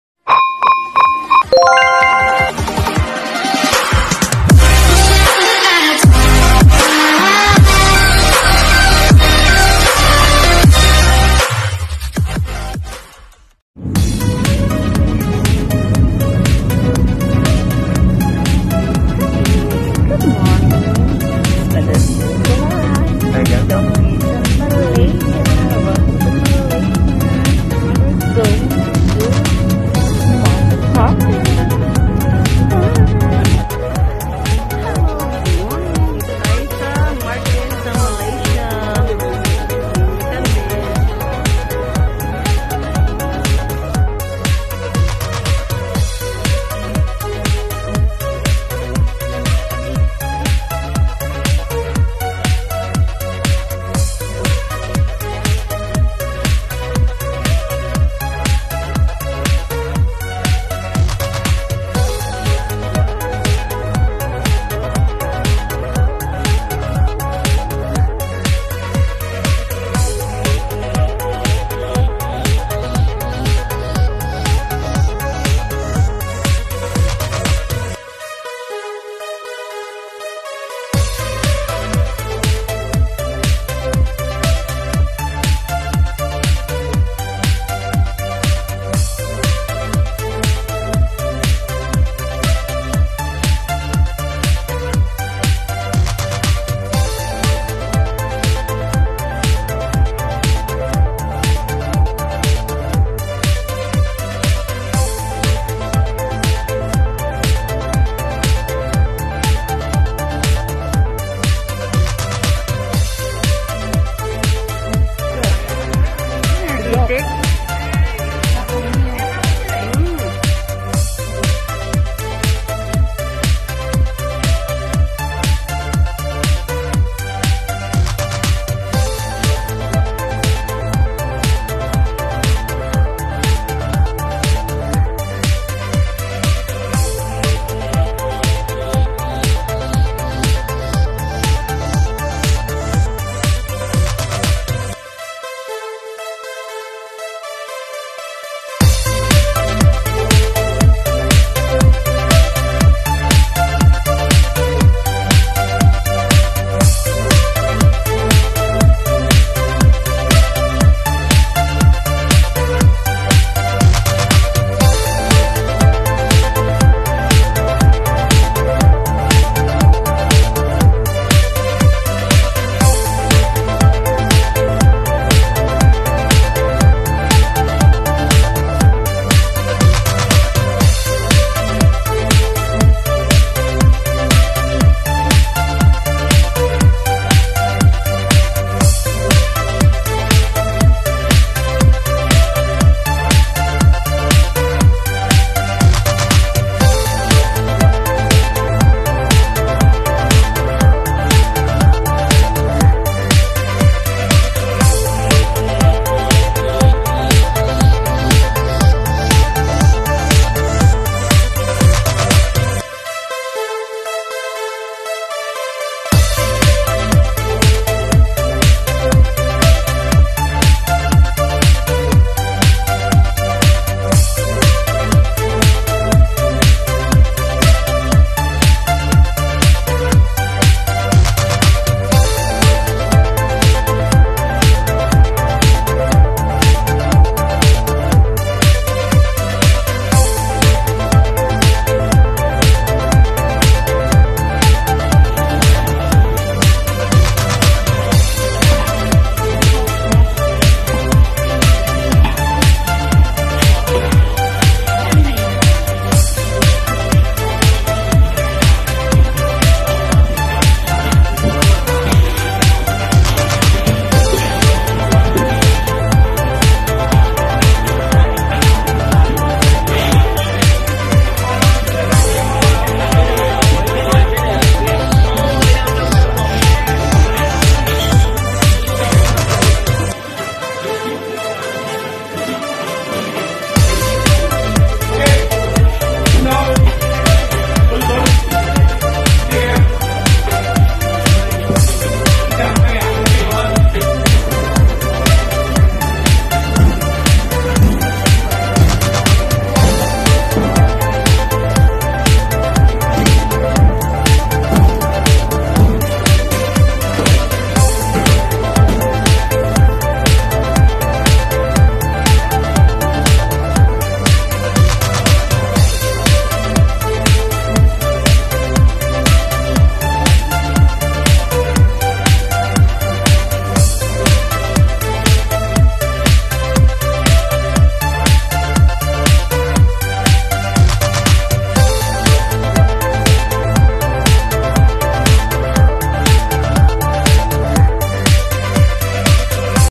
Market2 sa Malaysia
Its first time gala sa KL mamili sa Market2 Palengke sa Malaysia sa Panghanda sa Apo ko para sa Birthday nya Dami mura sarap mamili d2 😂 para dn pinas daming Tao